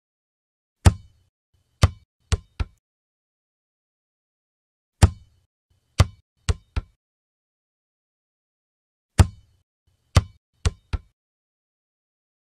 Line ball bouncing
Made in blender and the fed through linify.me.